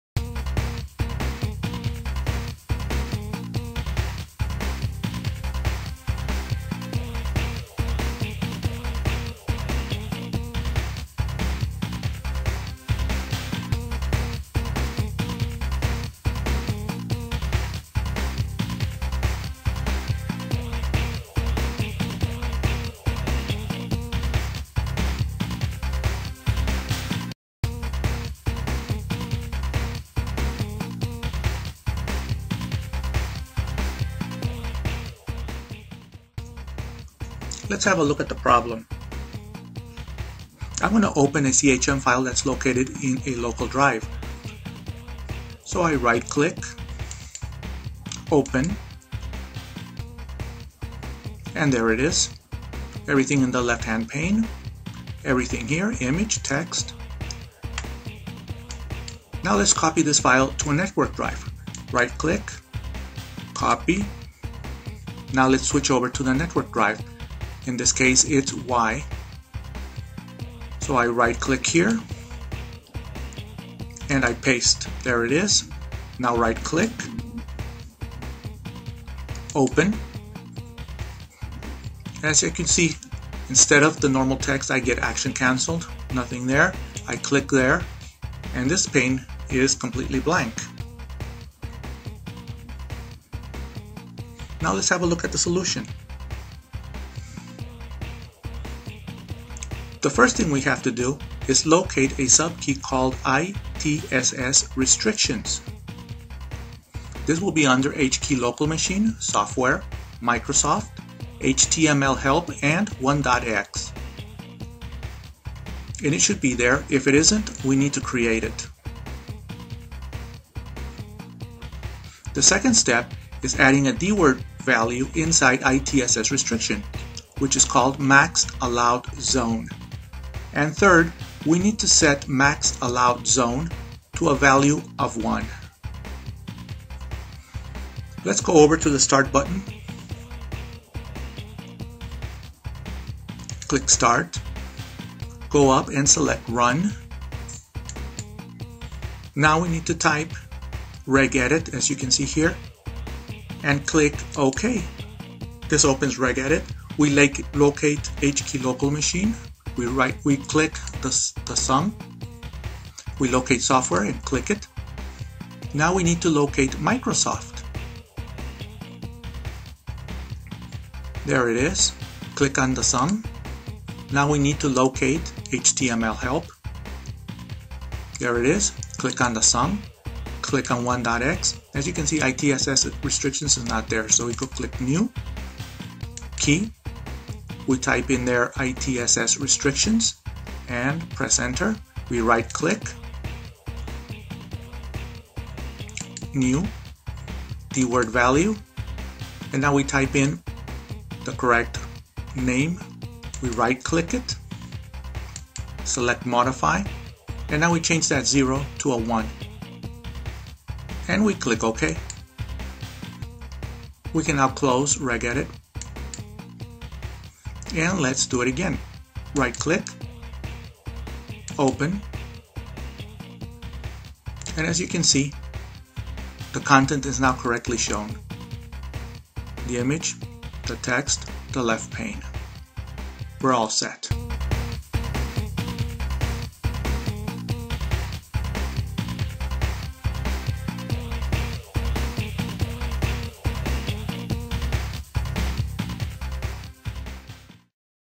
Viewing CHM files stored on the network
One possible solution to the issue of not being able to view CHM (compiled HTML Help) files that are stored on a network resource.